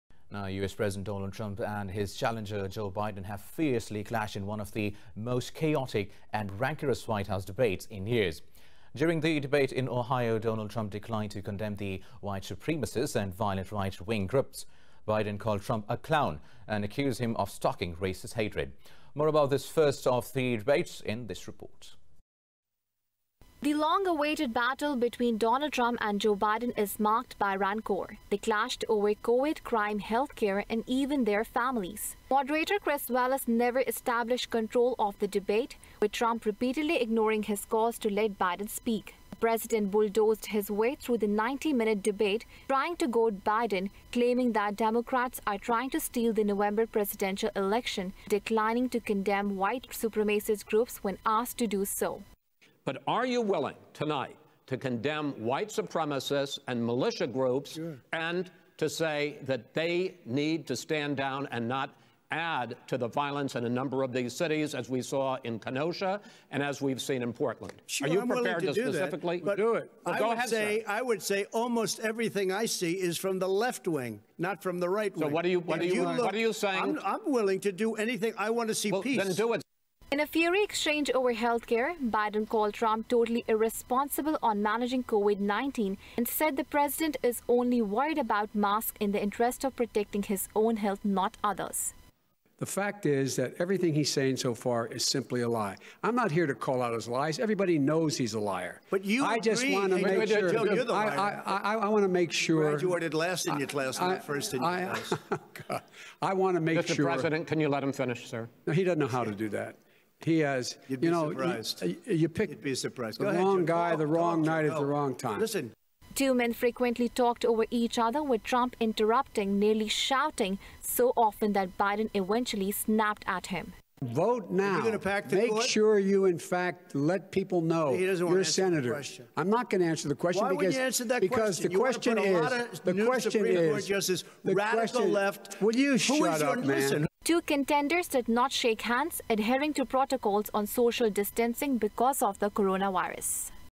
Race For White House: Trump, Biden Fiercely Clash In First Of Three Debates | News Bulletin
U.S. President DonaldTrump and his challenger JoeBiden have fiercely clashed in one of the most chaotic and rancorous White House debates in years.